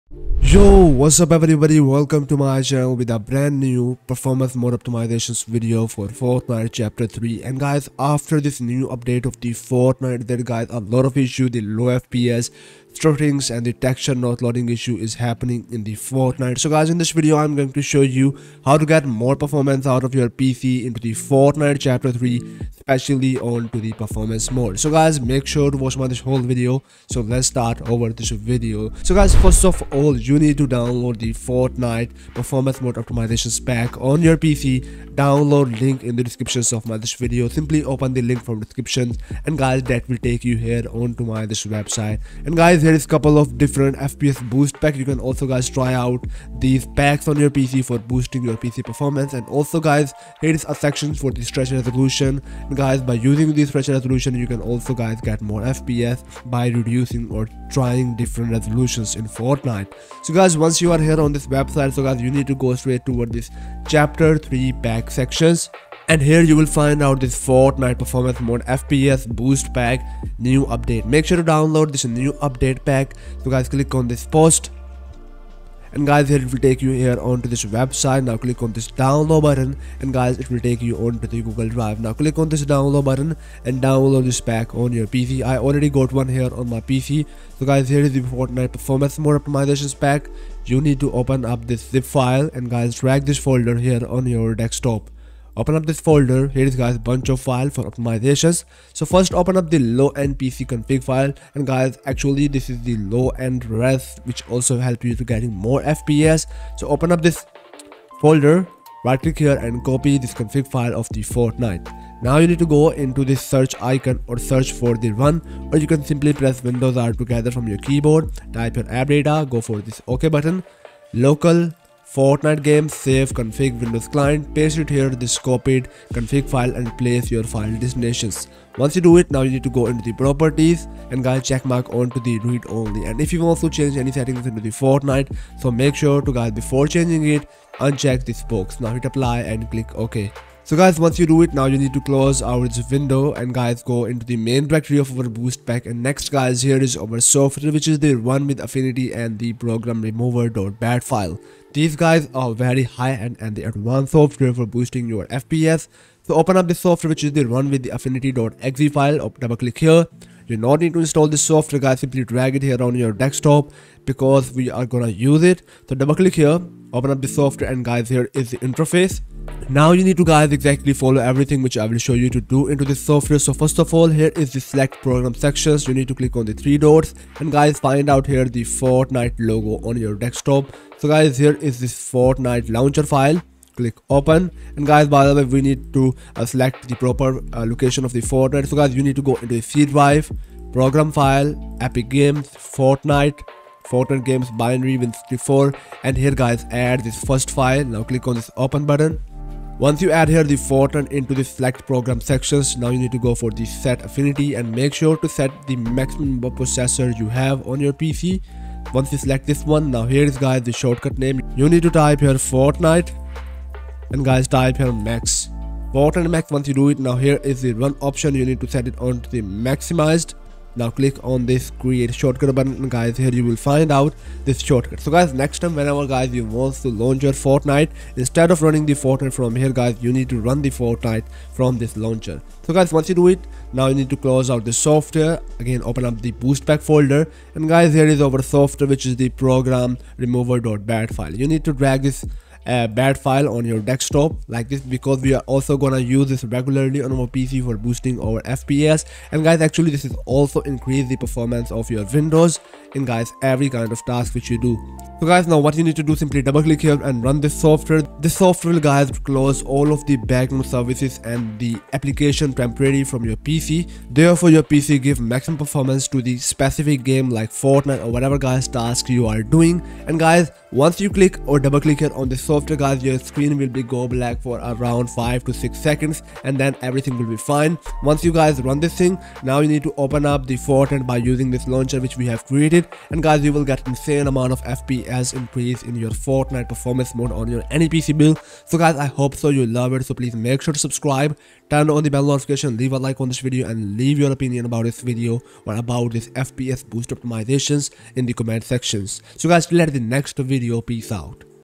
How To GET MAX FPS BOOST in Performance Mode! (Max FPS Boost Guide & 0 Input Delay)
Hello People!
Today in this video, I am showing you How To GET MAX FPS BOOST in Performance Mode! (Max FPS Boost Guide & 0 Input Delay) Which Gives You the Best Performance For Any PC Build. If You Wants To Boost Your FPS & Increase PC Performance So Make Sure To Watch Complete Video!

Title: How To GET MAX FPS BOOST in Performance Mode! (Max FPS Boost Guide & 0 Input Delay)